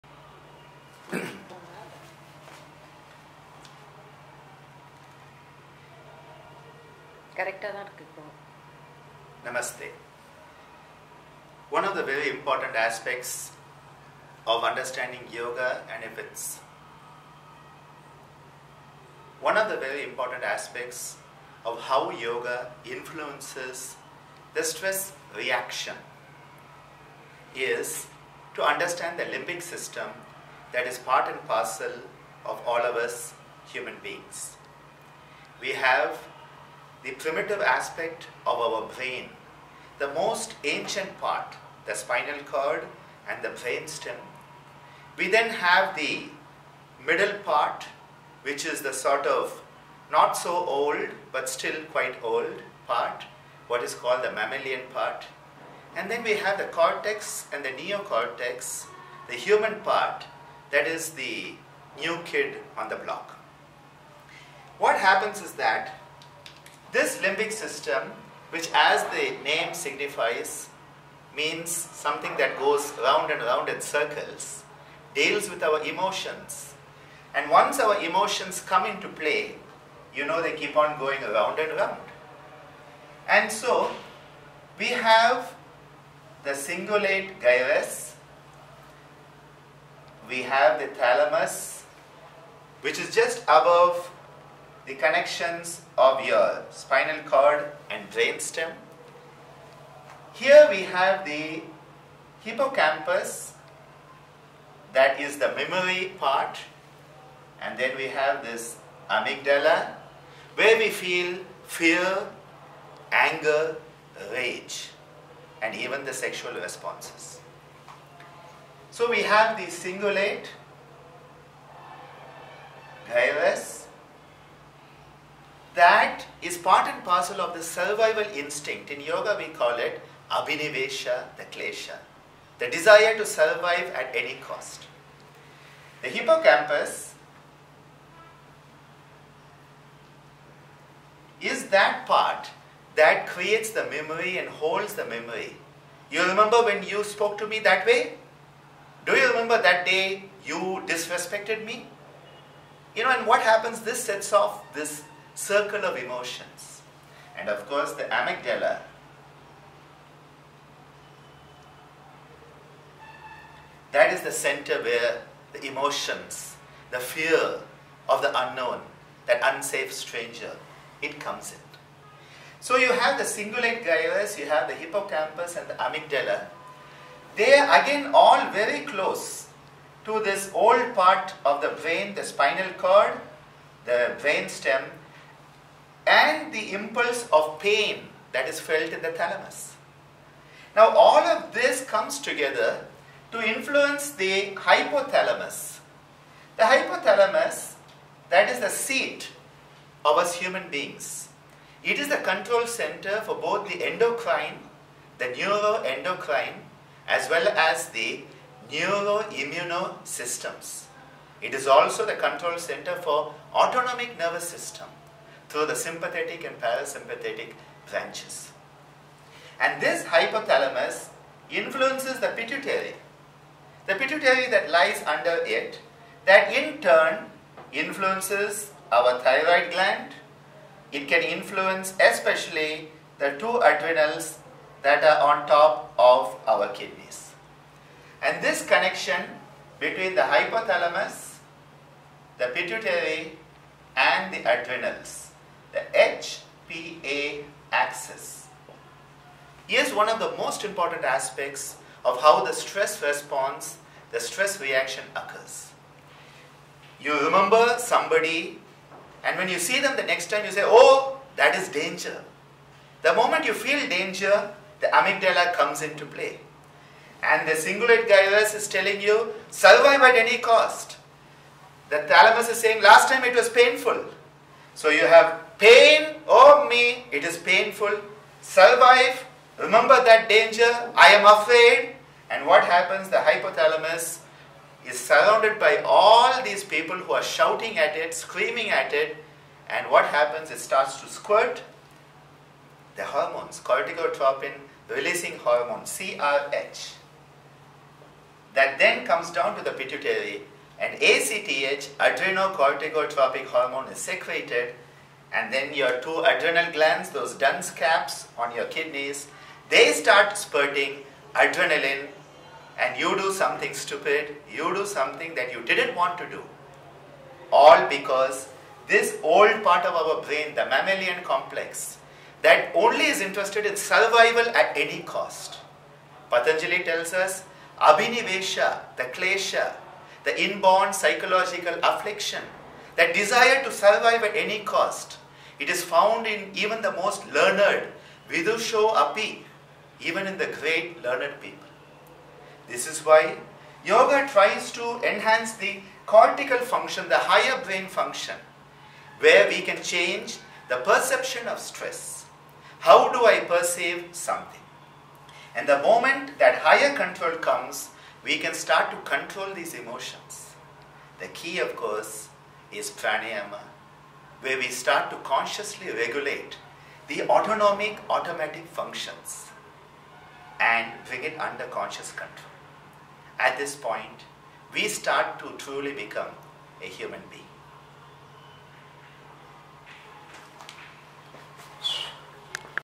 Understanding the limbic system and our stress reactions
Dr Ananda tries to briefly explain how our emotions and memories (limbic system) bring about stress reaction and how yoga may help.